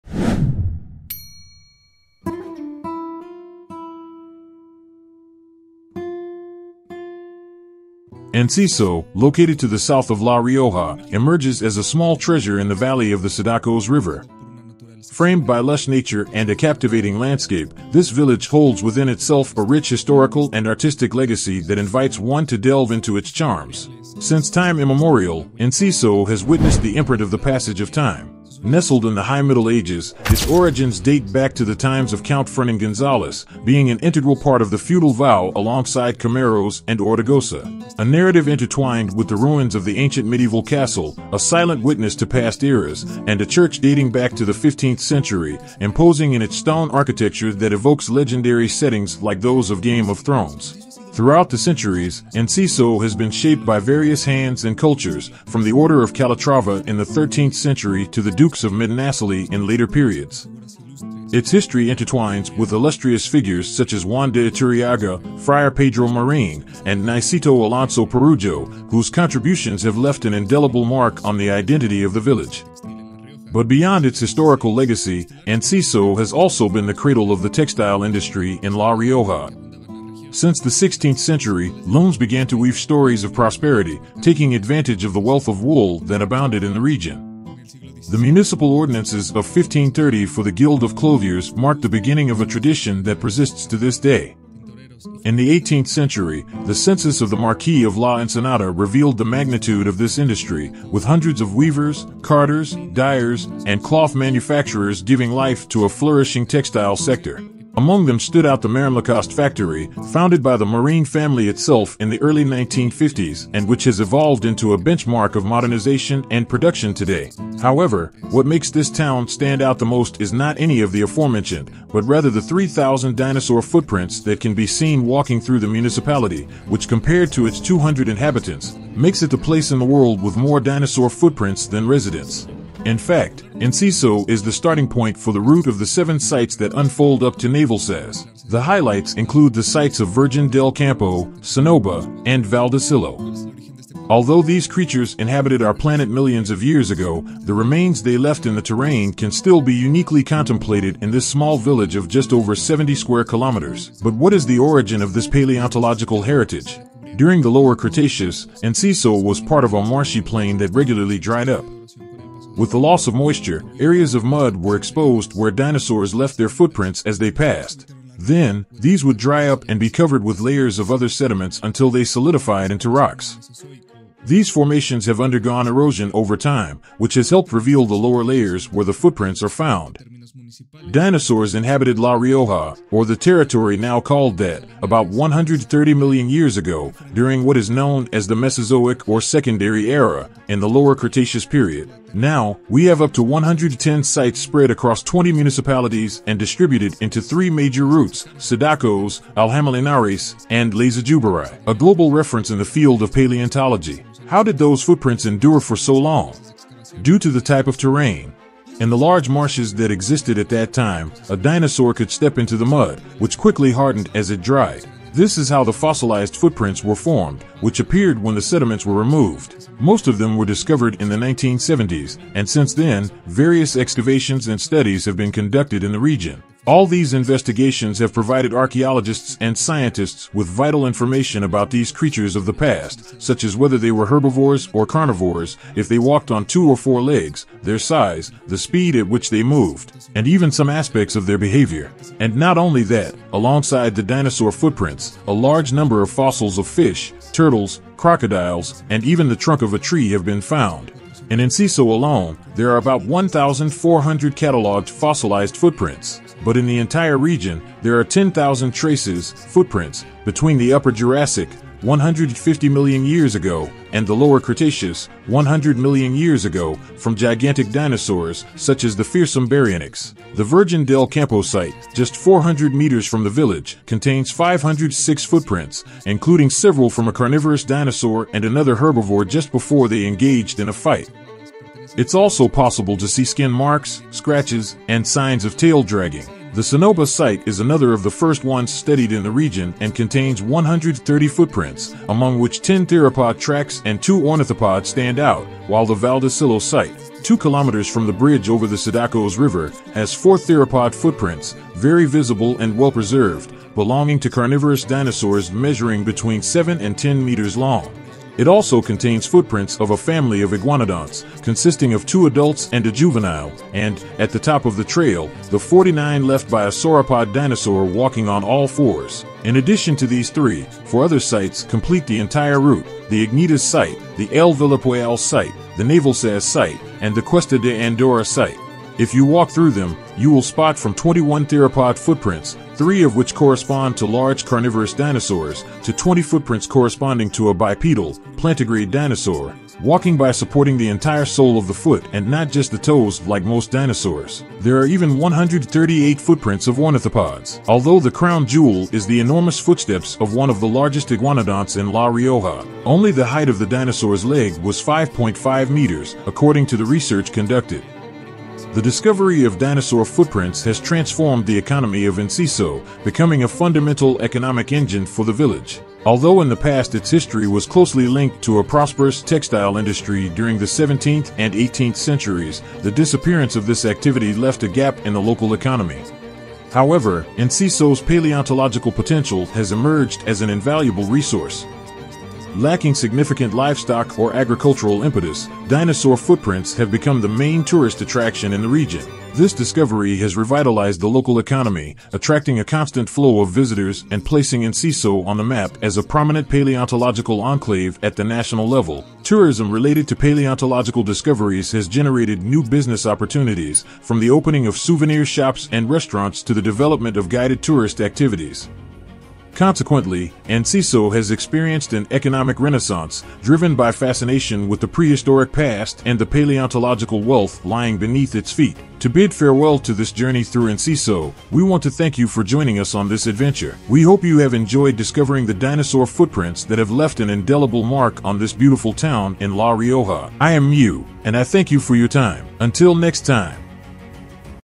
Enciso, the Spanish village with dinosaur footprints
Enciso, located to the south of La Rioja, emerges as a small treasure in the valley of the Cidacos River. Framed by lush nature and a captivating landscape, this village holds within itself a rich historical and artistic legacy that invites one to delve into its charms.